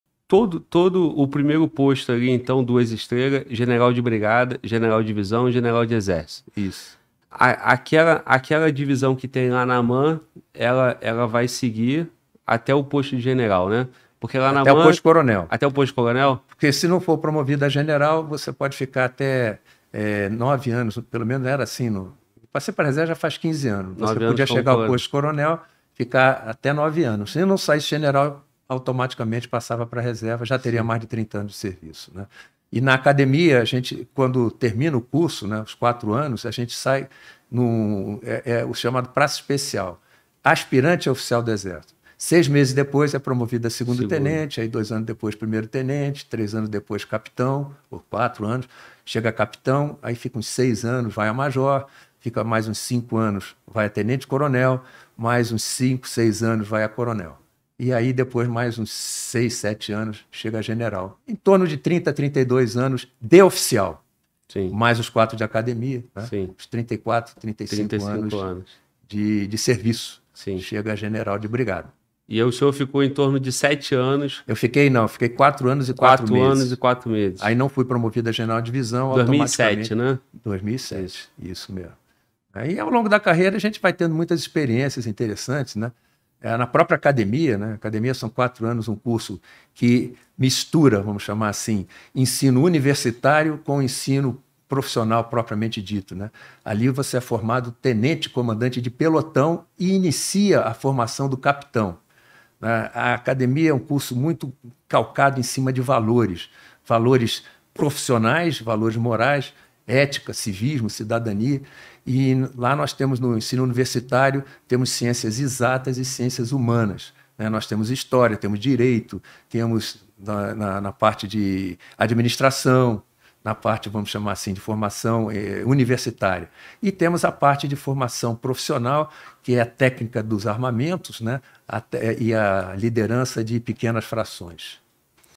QUANTO TEMPO DEMORA ATÉ SER GENERAL DO EXÉRCITO ?
FAAAAALAA!!! O PODCAST #149 É COM O GENERAL-DE-BRIGADA ROCHA PAIVA FORMADO NA AMAN EM 1973, POSSUI DIVERSAS CONDECORAÇÕES NACIONAIS E ESTRANGEIRAS.
NÃO PERCA! MAIS UM CONTÉUDO QUE VOCÊ ENCONTRA AQUI NO FALA GLAUBER PODCAST!!!

VIIIIIBRAAAAAAA!!!